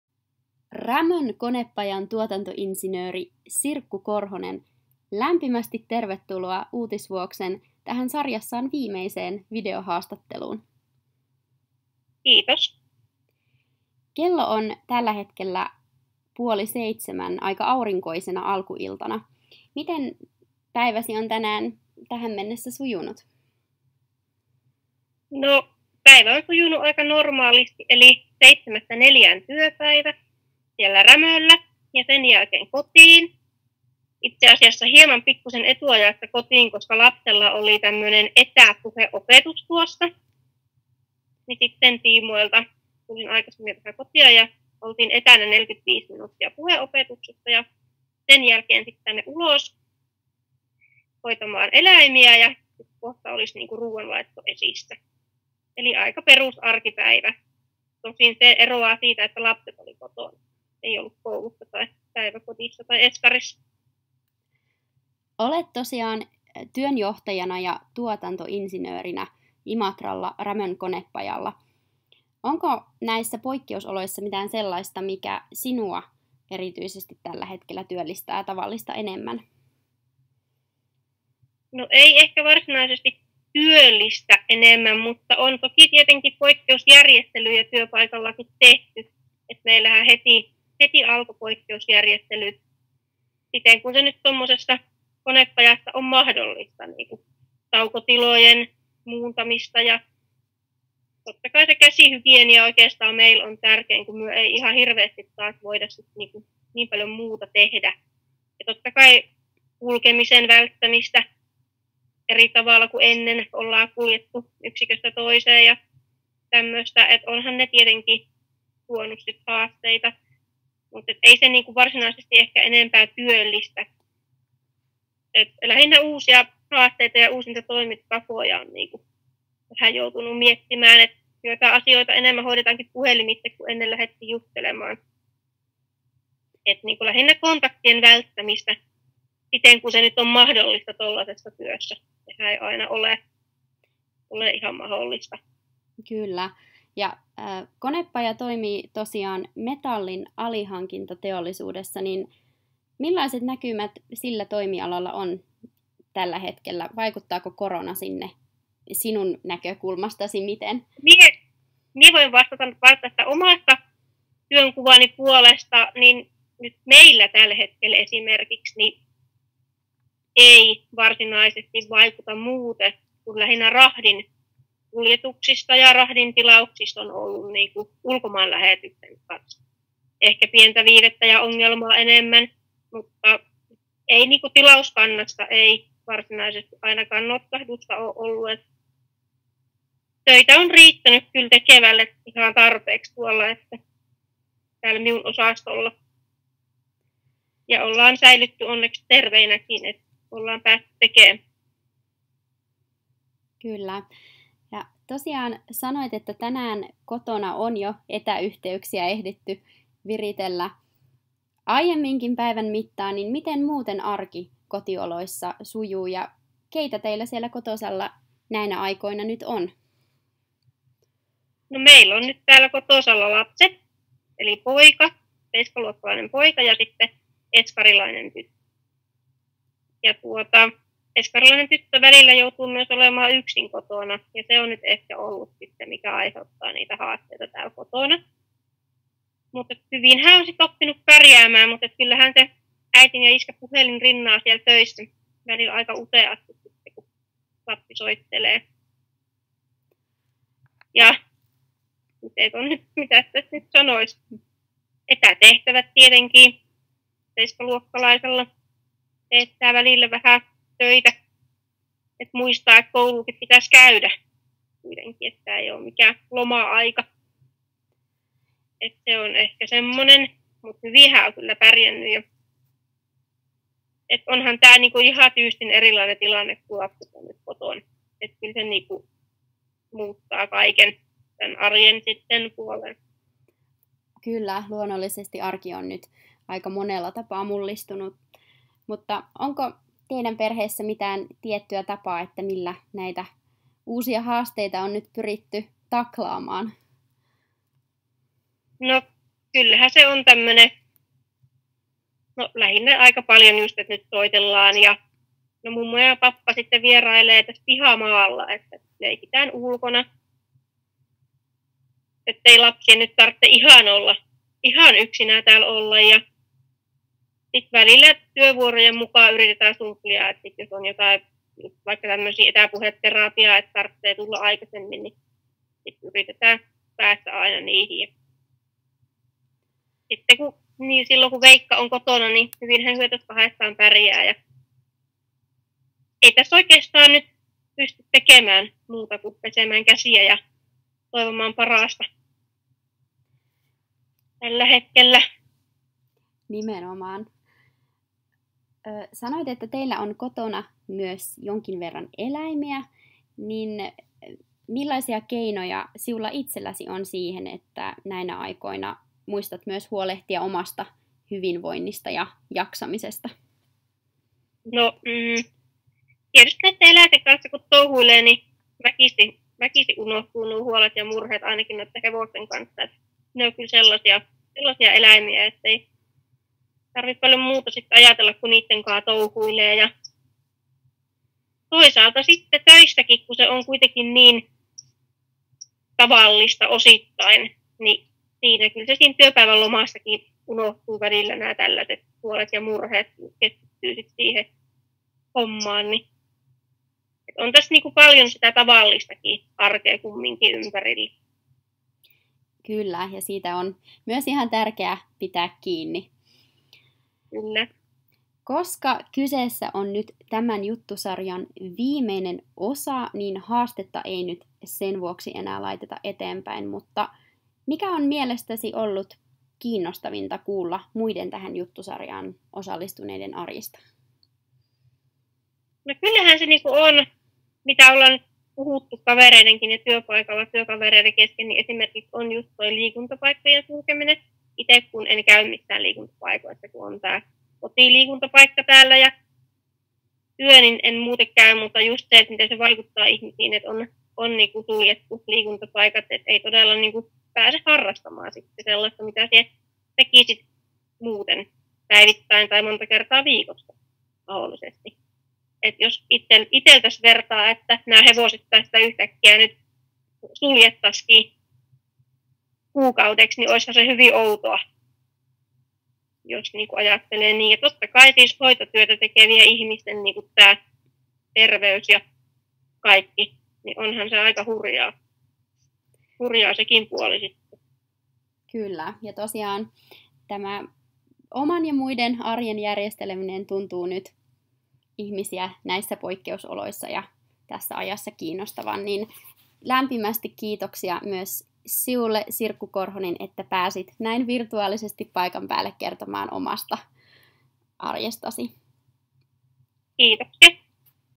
Arkea koronan aikana: Sirkku Korhosen videohaastattelu
Uutisvuoksen juttusarjassa imatralaisen Rämön konepajan tuotantoinsinööri ja työnjohtaja Sirkku Korhonen kertoo korona-ajan arjesta. Haastattelijana äänessä Jasmin Ojalainen.